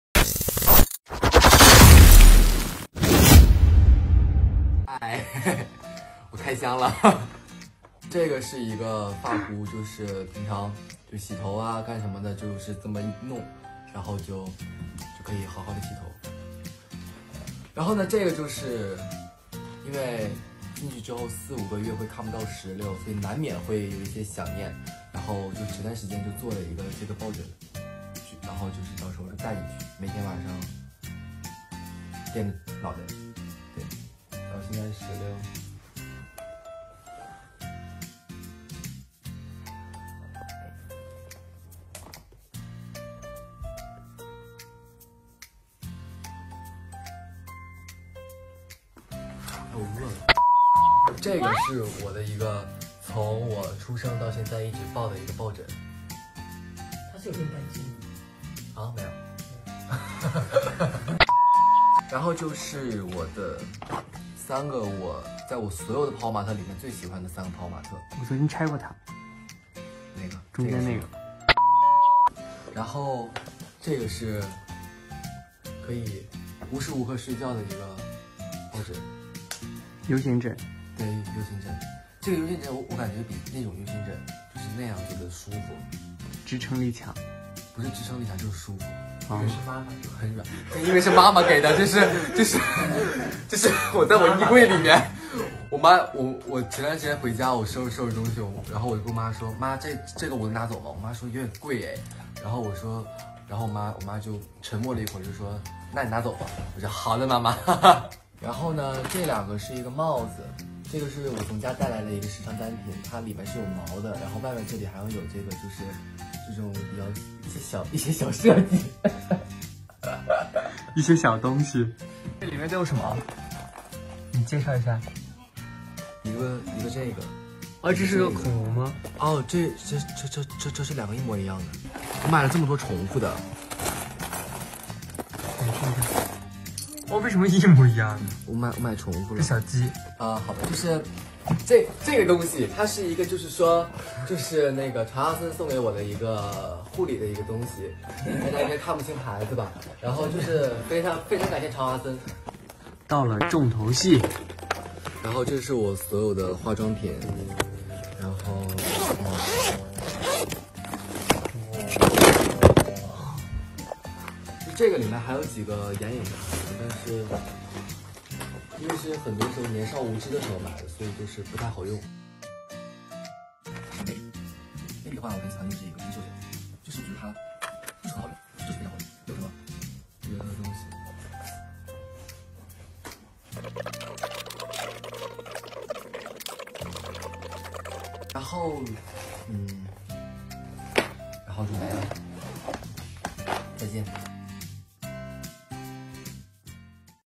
[ENG SUB] Packing for Dachang with Zhang Shuaibo
Haohan trainee Zhang Shuaibo preparing for Youth With You Season 3

Please support Zhang Shuaibo on the upcoming Boy's Planet survival show!